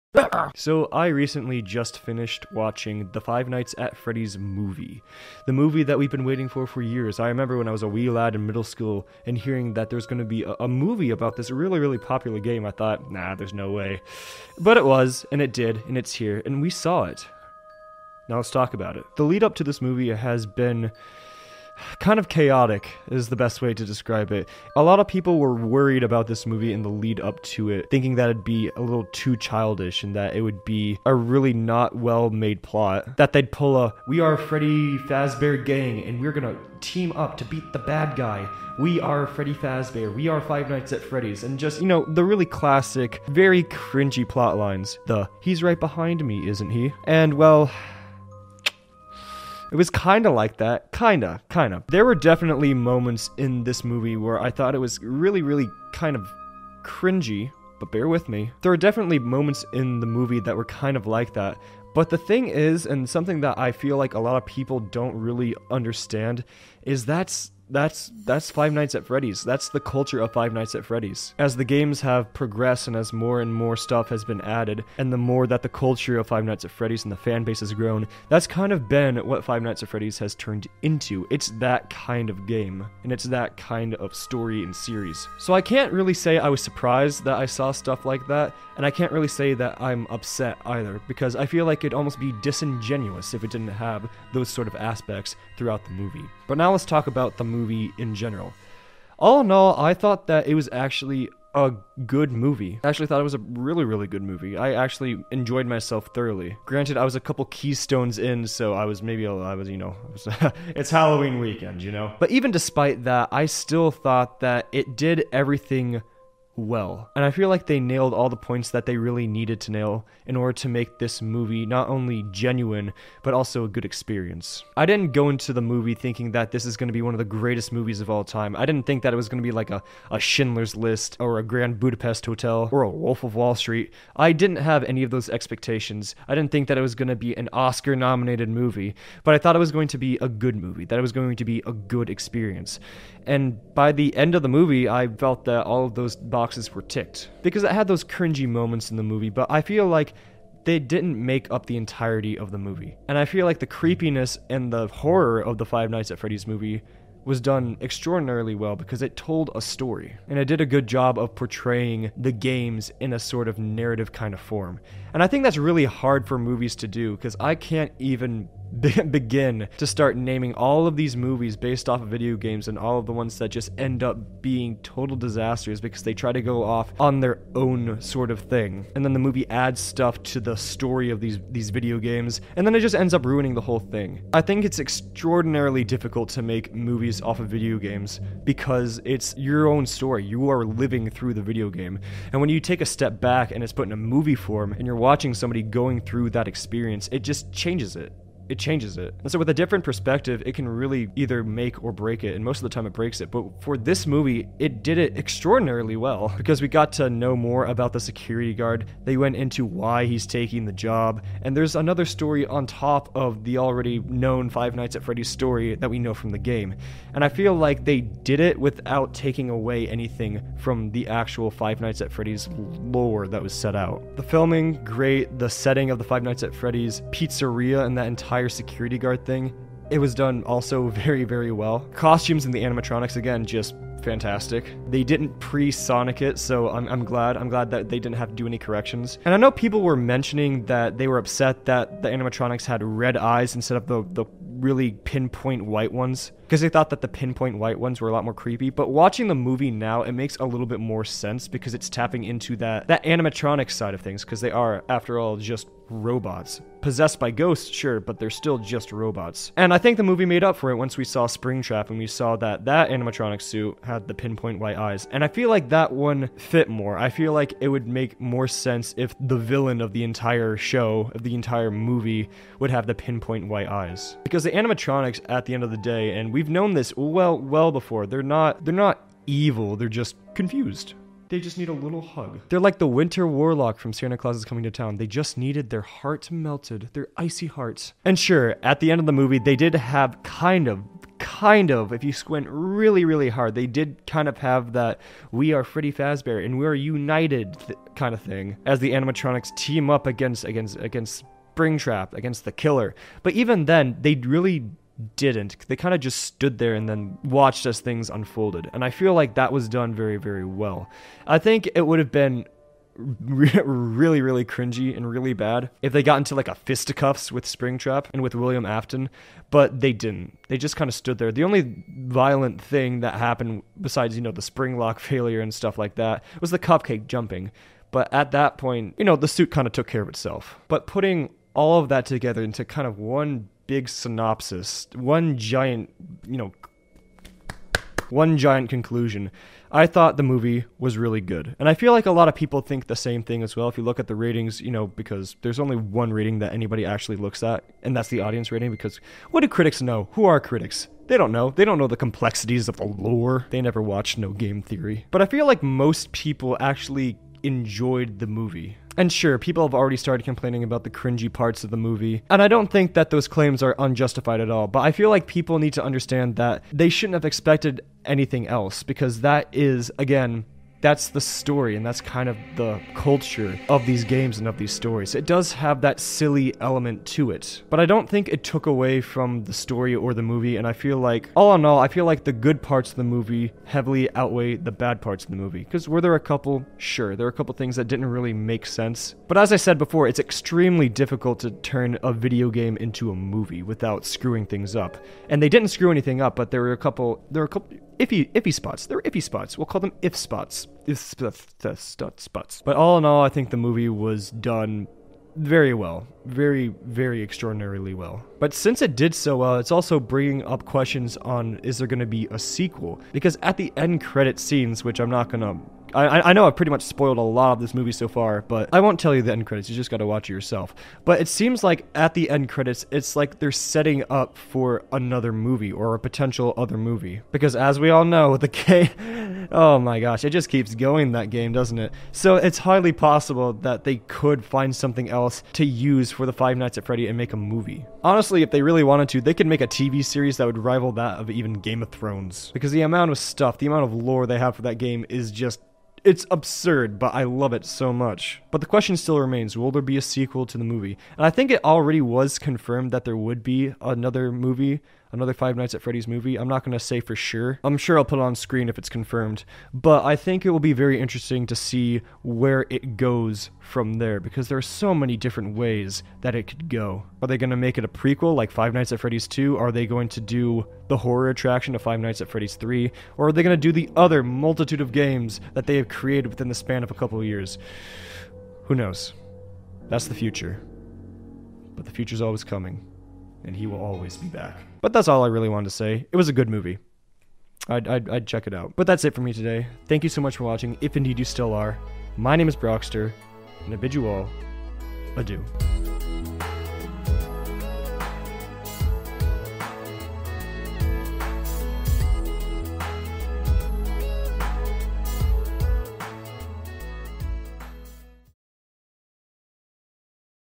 Five Nights At Freddy's Movie SHOCKING TRUTH!
With the new Five Nights At Freddy's movie, was it good? What is the shocking truth behind this film? Will there be another one?